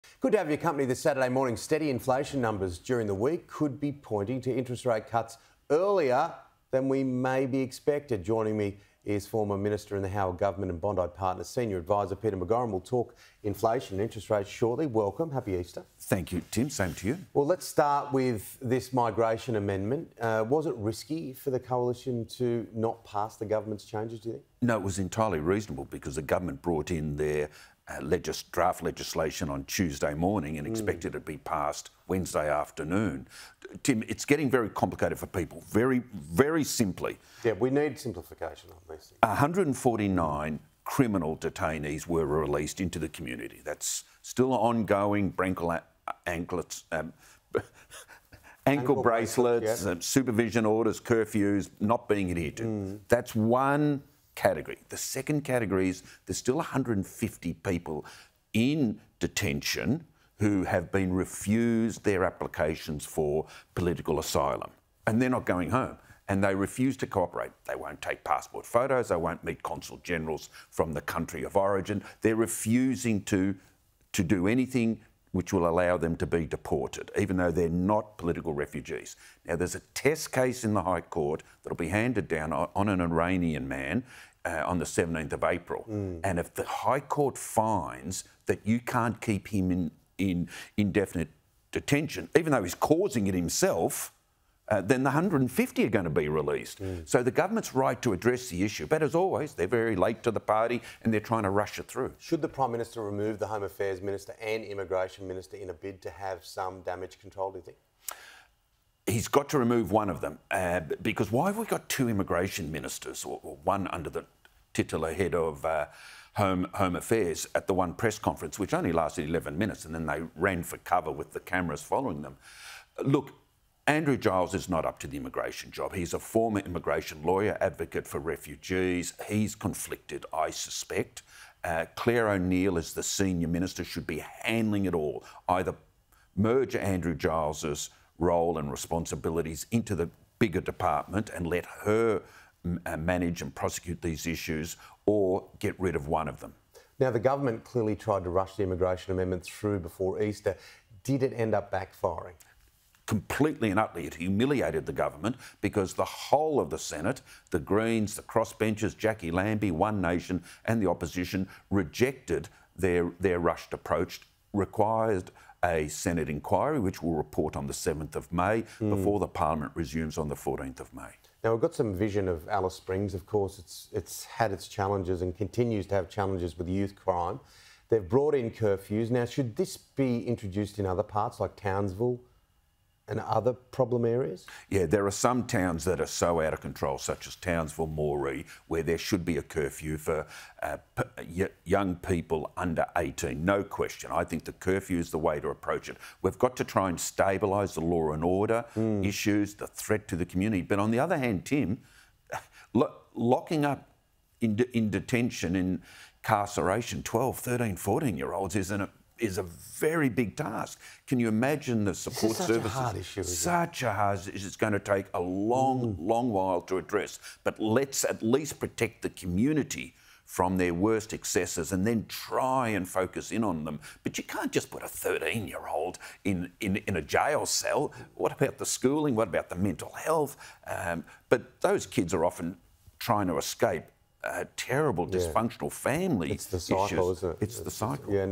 ‘The curfew is the way’: Peter McGauran on how to manage ‘out of control’ towns
Bondi Partners Senior Advisor Peter McGauran believes a curfew is the “way to approach” crime in “out of control” towns.

Northern Territory Chief Minister Eva Lawler has declared an ‘emergency situation’ in the Alice Springs CBD to combat crime.

A two-week youth curfew will be put in place as part of the response.

“There are some towns that are so out of control like Townsville, Moree, where there should be a curfew for young people under 18 – no question,” Mr McGauran told Sky News Australia.

“I think the curfew is the way to approach it.

“We’ve got to try to stabilise the law-and-order issues, the threat to the community.”